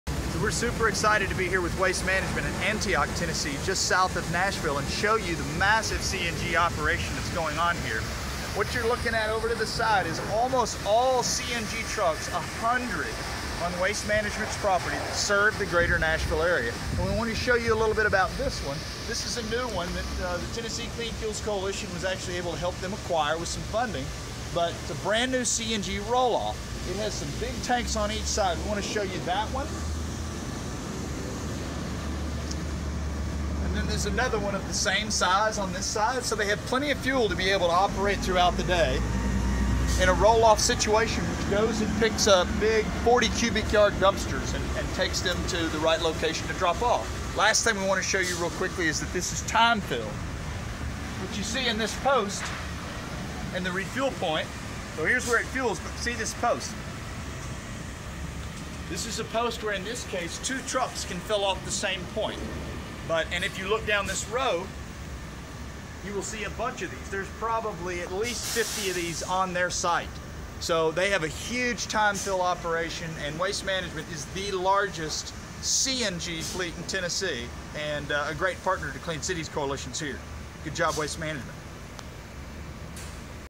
TNCleanFuels checks out Waste Management's amazing CNG fleet in Nashville, TN
The East Tennessee Clean Fuels Coalition partners with the Tennessee Dept. of Environment and Conservation to manage the allotment of state EPA funding that TN receives each year through the “Reducing Diesel Emission for a Healthier Tennessee” (RDE4HT) Rebate Program.

Waste Management has applied for and received funding to expand their fleet of CNG trucks in Tennessee, including in locations where they are building out their CNG fleet, like in Jackson and Knoxville, Tennessee. The roll off truck that is shown in the video is just one of over 10 trucks that they have added recently into their fleet via the RDE4HT Program. Way to go WasteManagement!!